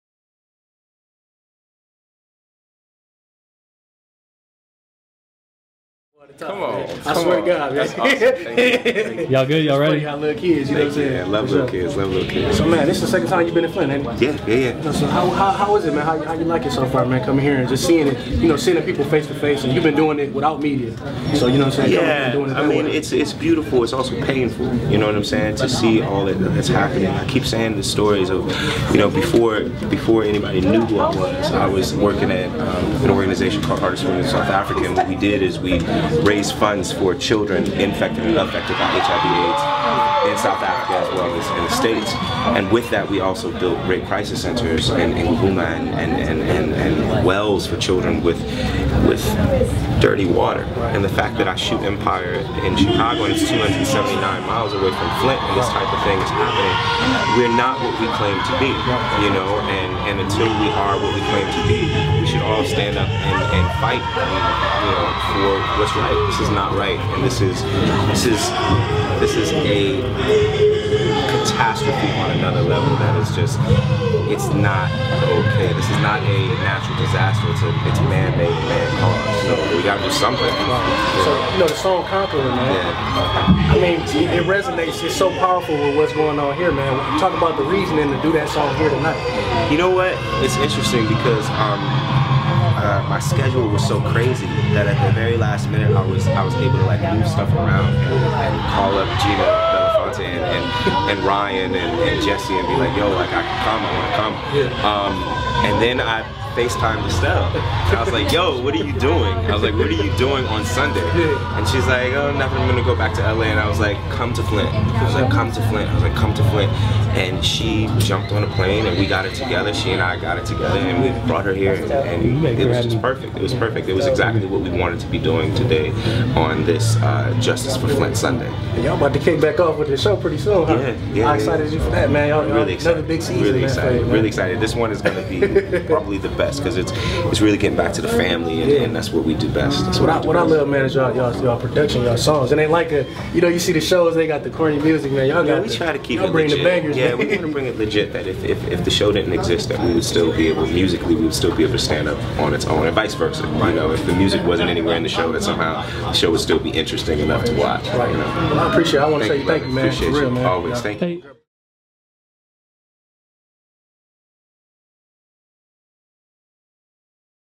Empire star Jussie Smollett talks new season, Flint water crisis
Empire star Jussie Smollett is best known for his role as Jamal Lyon on the hit show. Smollett was in town among many stars at the Whiting Auditorium on Sunday, Feb. 28 for Revolt TV’s Justice For Flint event. Smollett talks to reporter Eric Woodyard about the Flint water crisis, the new season of Empire and his music.